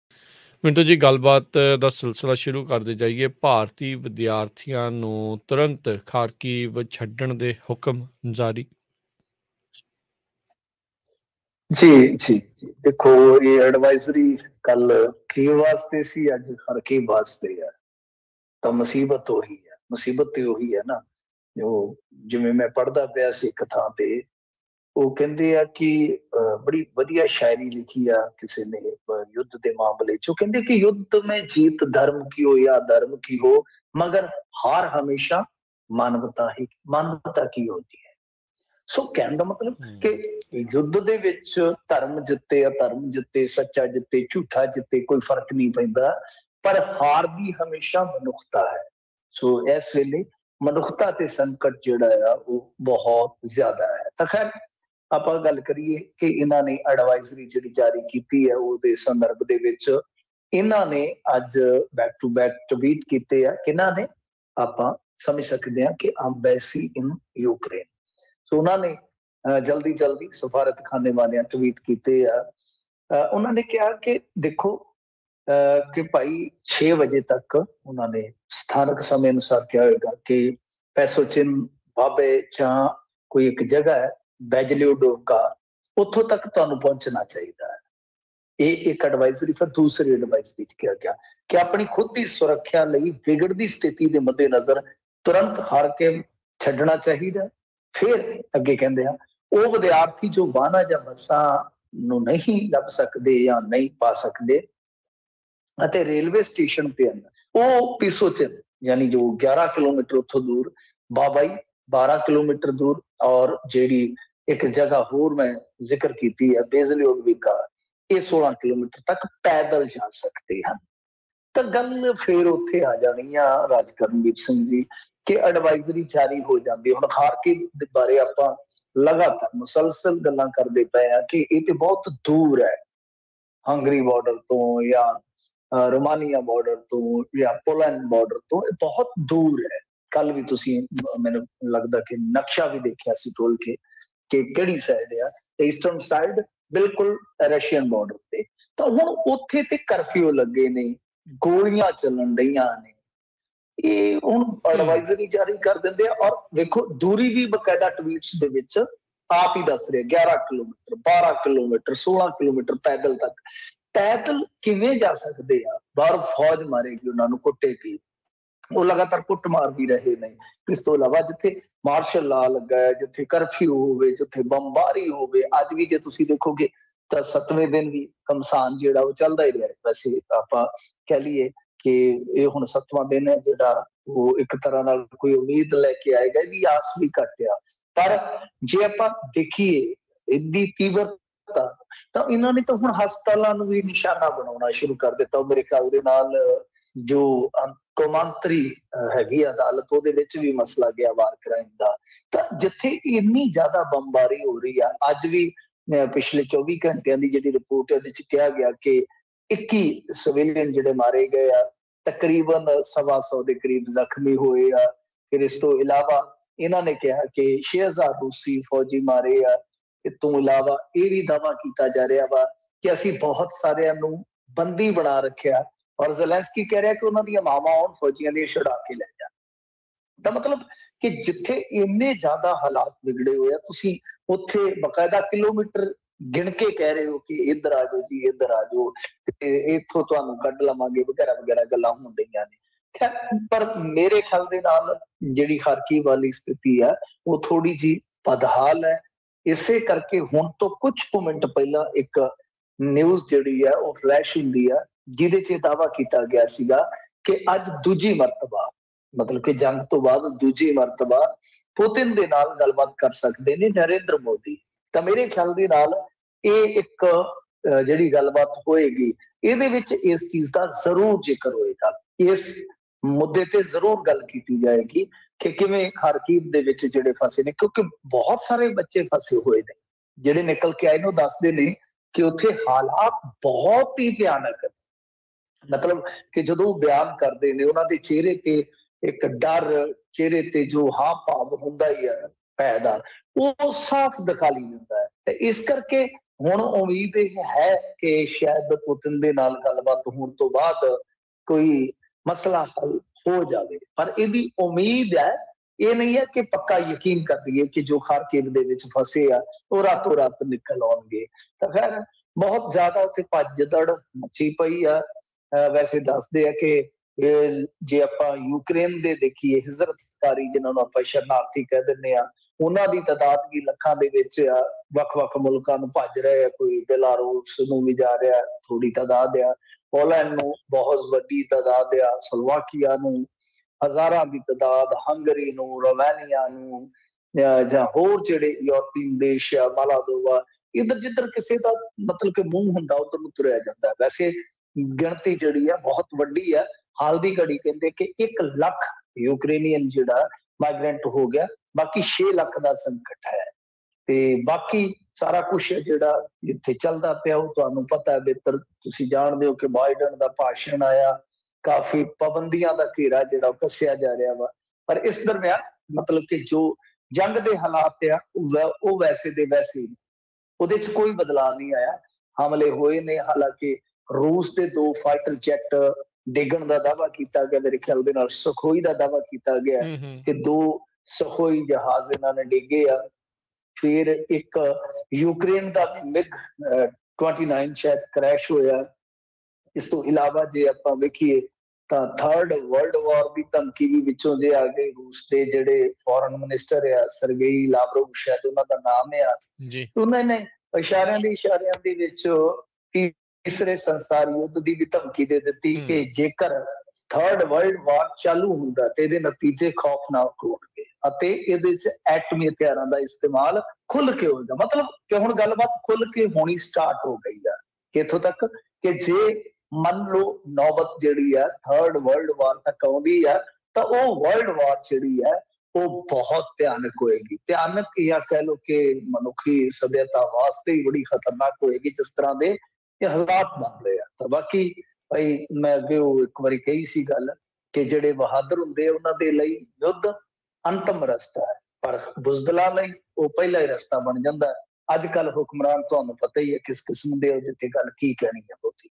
ਯੂਕਰੇਨ 'ਚ ਫਸੇ ਭਾਰਤੀਆਂ ਲਈ ਨਵੀਂ ADVISORY ਜਾਰੀ ਹੁਣ ਕੀ ਕਰਨ ਵਿਦਿਆਰਥੀ ?
ਯੂਕਰੇਨ 'ਚ ਫਸੇ ਭਾਰਤੀਆਂ ਲਈ ਨਵੀਂ ADVISORY ਜਾਰੀ
ਹੁਣ ਕੀ ਕਰਨ ਵਿਦਿਆਰਥੀ ?